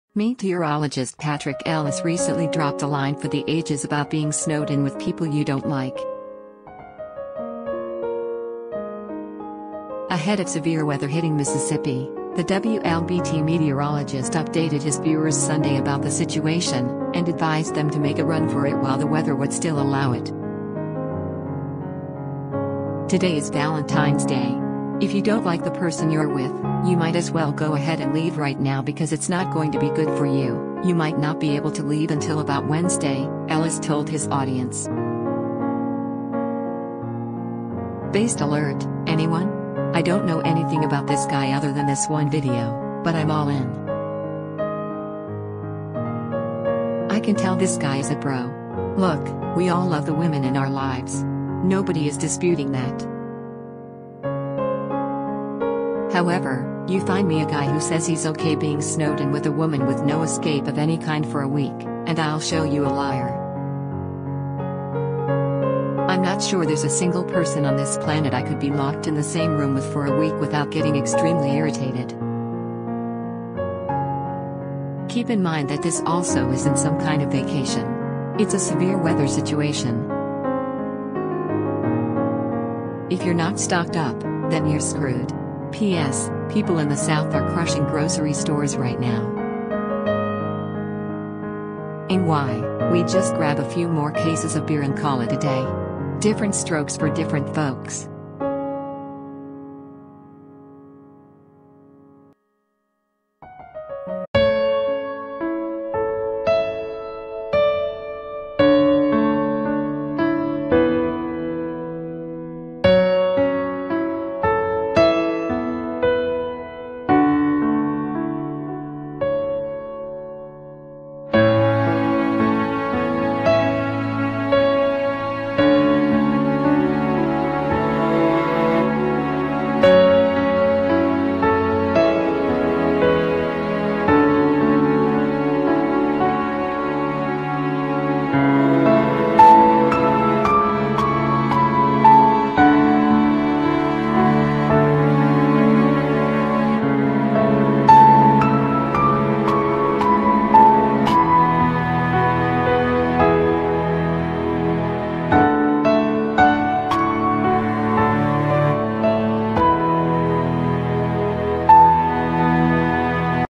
Meteorologist Patrick Ellis Drops Hilarious Line About Being Snowed In With People You Don’t Like
Meteorologist Patrick Ellis recently dropped a line for the ages about being snowed in with people you don’t like.  Ahead of severe weather hitting Mississippi, the WLBT meteorologist updated his viewers Sunday about the situation, and advised them to make a run for it while the weather would still allow it.   “Today is Valentine’s Day. If you don’t like the person you’re with, you might as well go ahead and leave right now because it’s not going to be good for you…You might not be able to leave until about Wednesday,” Ellis told his audience.    Based alert, anyone? I don’t know anything about this guy other than this one video, but I’m all in. I can tell this guy is a bro.   Look, we all love the women in our lives. Nobody is disputing that. However, you find me a guy who says he’s okay being snowed in with a woman with no escape of any kind for a week, and I’ll show you a liar.   I’m not sure there’s a single person on this planet I could be locked in the same room with for a week without getting extremely irritated.   Keep in mind that this also isn’t some kind of vacation. It’s a severe weather situation. If you’re not stocked up, then you’re screwed.  P.S.: People in the south are crushing grocery stores right now. In WI, we’d just grab a few more cases of beer and call it a day. Different strokes for different folks.